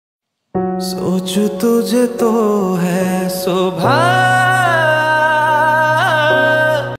SILVER 92.5 CHAND NECKLACE
Love ! This one is ethereal! The sparkling kundan stones against your skin  . Made in pure 92.5 silver.